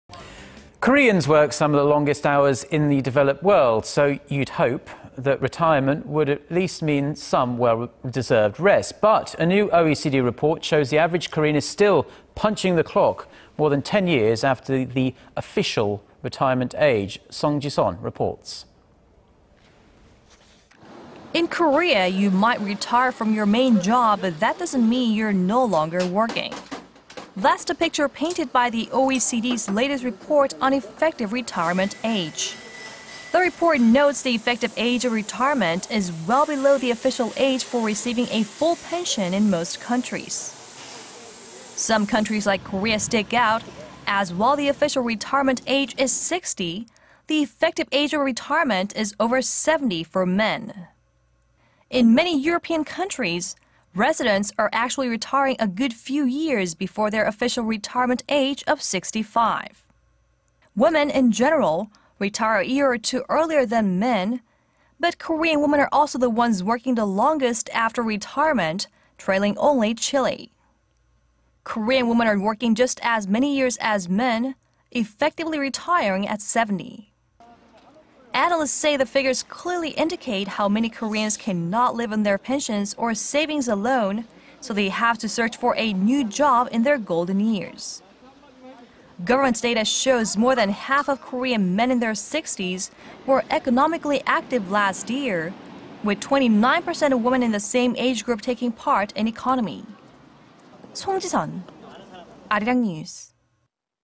Koreans work longer after retirement: OECD
Koreans work some of the longest hours in the developed world, so you'd hope retirement would mean some well-deserved rest.
But a new OECD report shows the average Korean is still punching the clock... more than 10 years after the "official" retirement age.
Song Ji-sun reports.
In Korea, you might retire from your main job but that doesn't mean you are no longer working.
That's the picture painted by the OECD's latest report on effective retirement age.
The report notes the effective age of retirement is well below the official age for receiving a full pension in most countries.
Some countries like Korea stick out as while the official retirement age is 60, the effective age of retirement is over 70 for men.
In many European countries, residents are actually retiring a good few years before their official retirement age of 65.
Women, in general, retire a year or two earlier than men... but Korean women are also the ones working the longest after retirement... trailing only Chile.
Korean women are working just as many years as men -- effectively retiring at 70.
Analysts say the figures clearly indicate how many Koreans cannot live on their pensions or savings alone so they have to search for a new job in their golden years.
Government data shows more than half of Korean men in their 60s were economically active last year... with 29-percent of women in the same age group taking part in economy.